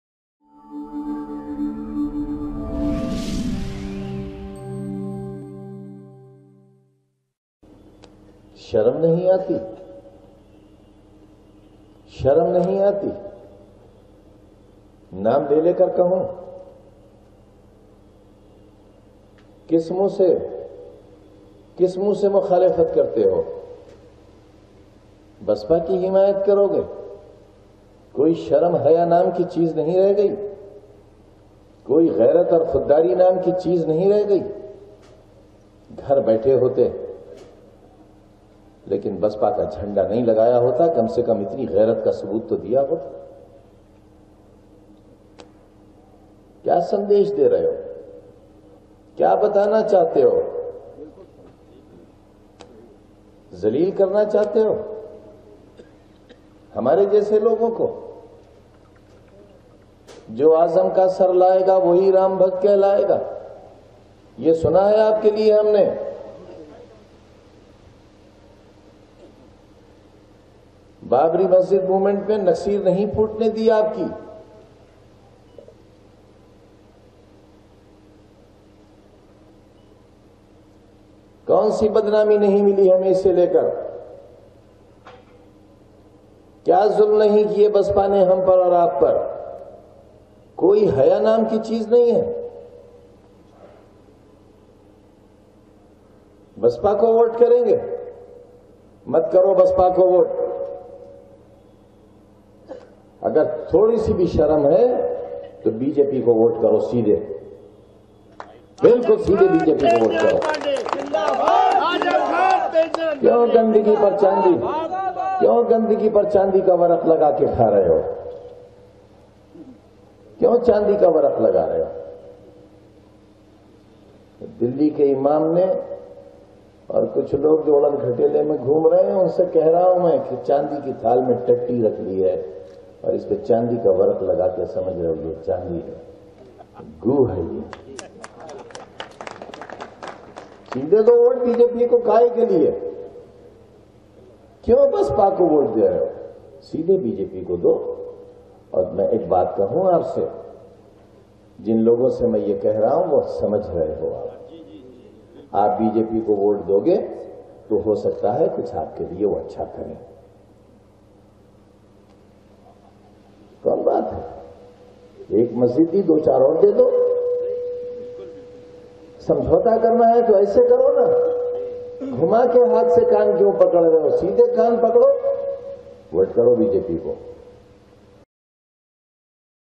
आजम खाँ को क्यों कहना पड़ा बीजेपी को ओट दो ,क्या आजम खाँ बीजेपी में होगें शामिल
NioTimes is India's best Hindi News Channel.NioTimes news channel covers latest news in politics, entertainment, bollywood, business and sports.